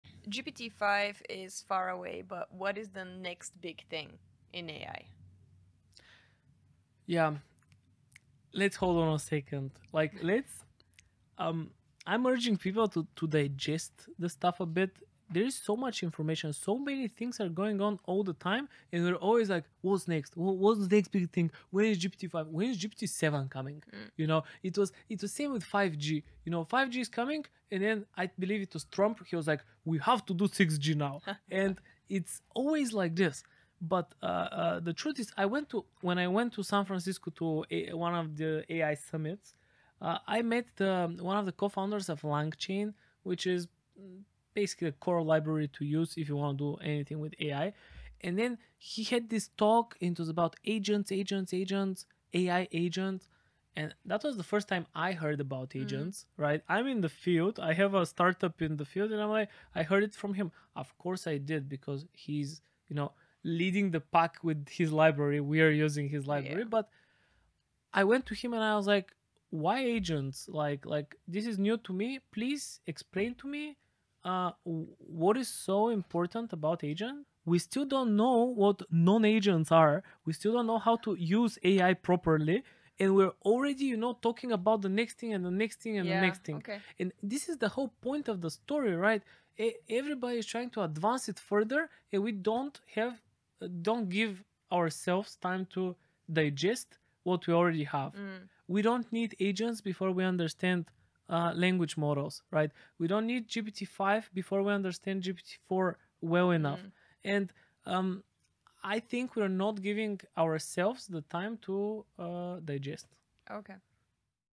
Why We Are Not Ready for GPT-4
Are you eagerly anticipating GPT-5? In today's conversation, we talk about the constant need of people for the new and improved. Amidst the race for advancements, let's slow down and truly understand the power of AI. Before we rush towards GPT-5, have we fully understood GPT-4 yet? We're urging you to digest, embrace, and explore AI at your own pace.